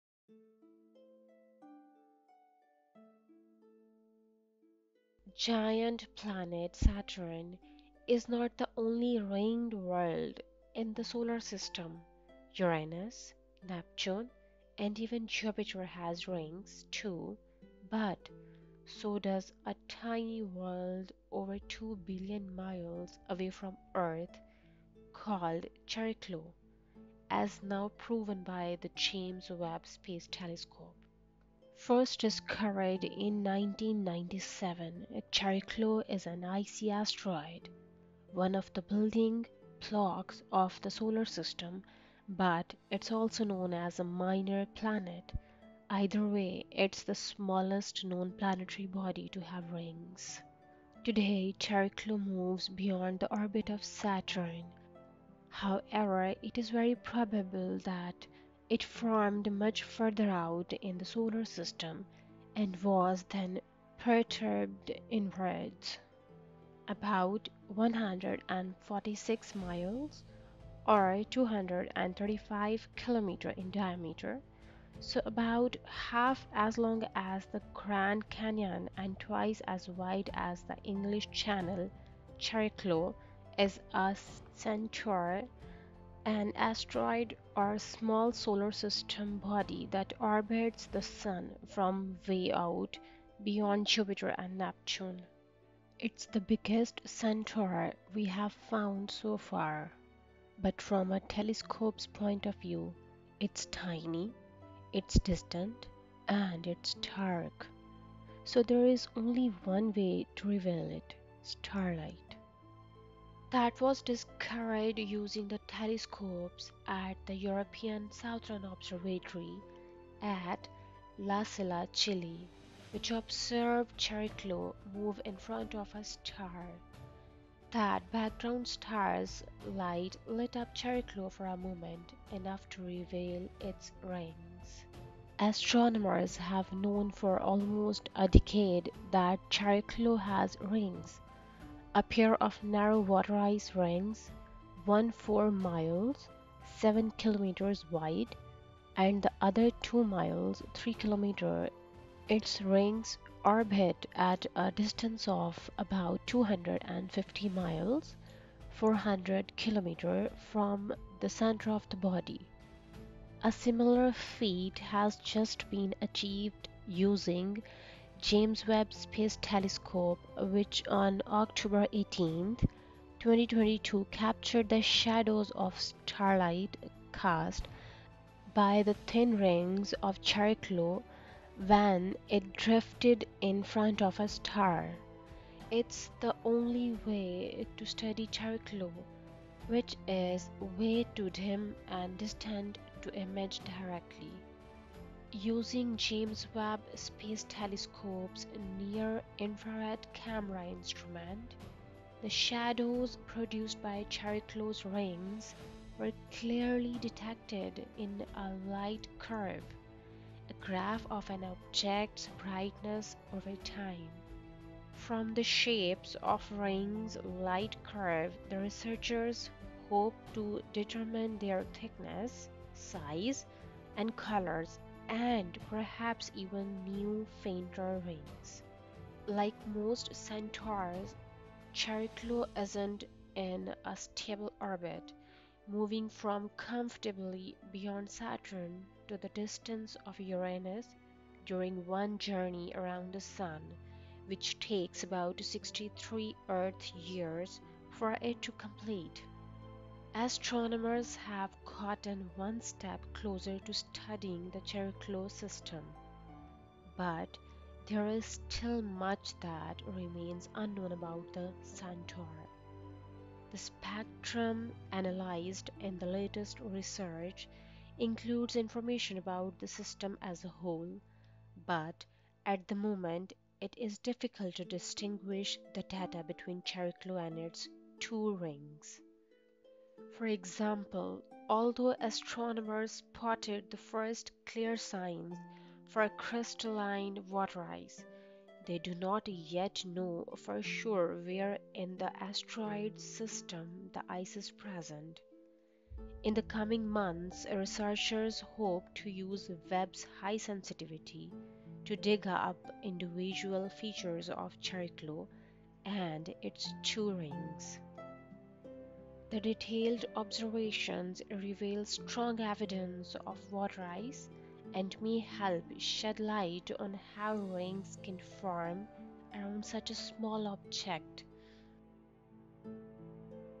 Chariklo | The First Ever Asteroid World With Rings Just Examined by The James Webb Space Telescope
The asteroid Chariklo has been confirmed as the smallest object in the Solar System to display a ring system. James Webb Space Telescope discovers water ice at ringed asteroid Chariklo 'by remarkable luck'.